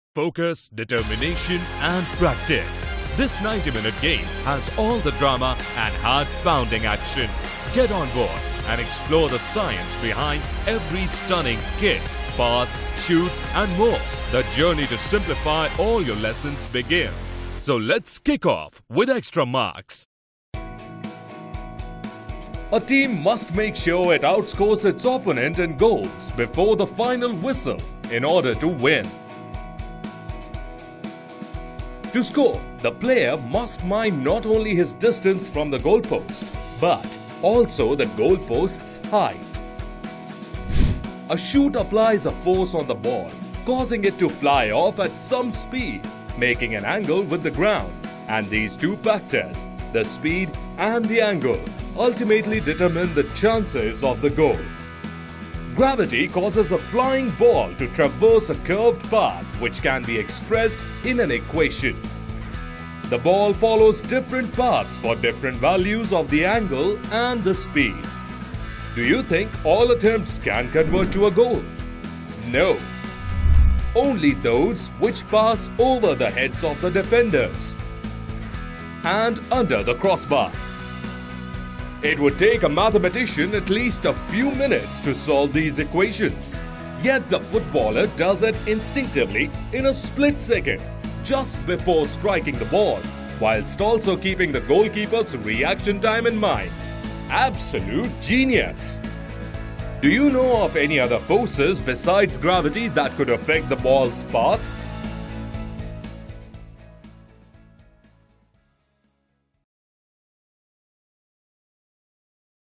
Kick-Off | Extramarks x Arsenal | Ep 08: Guided by Gravity
Join us as we take a closer look at the science behind football with our latest episode of Kick-Off in association with Arsenal FC. 'Guided by Gravity' explores the effect of gravitational forces on the game and how it plays a crucial role in everything from the trajectory of the ball to the movements of the players. Get a unique insight into the sport and learn something new with Extramarks Education and Arsenal FC.